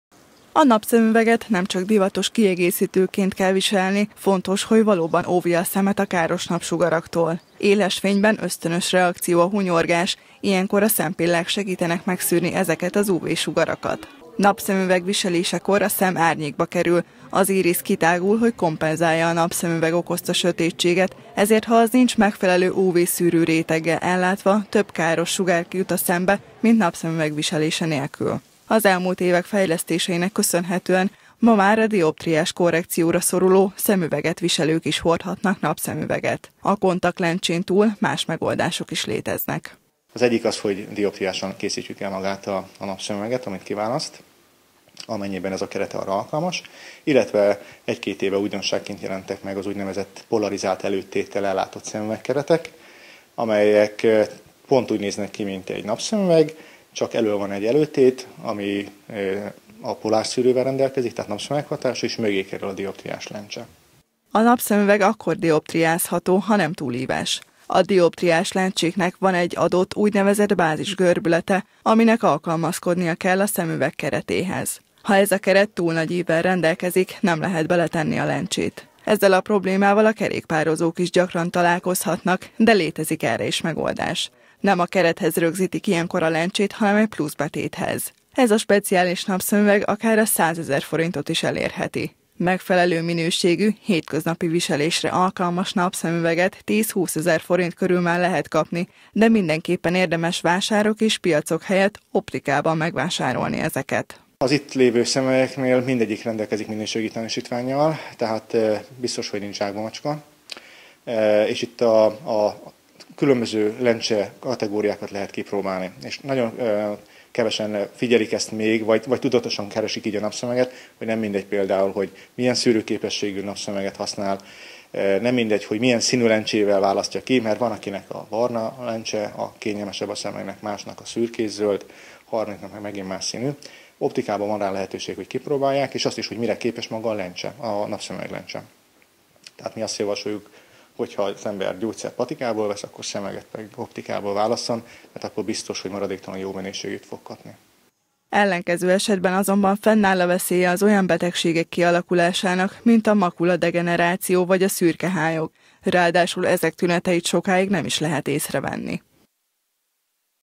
Fontos a jó napszemüveg - Kölcsey Tv
A hazai lakosság mintegy fele hord napszemüveget, még kevesebben a megfelelő UV-szűrővel rendelkezőt. Sokan nem a minőség, hanem a forma és szín alapján választanak napszemüveget, amivel többet ártanak a szemnek, mint amennyit használnak. Így az évek során akár szürkehályog is kialakulhat.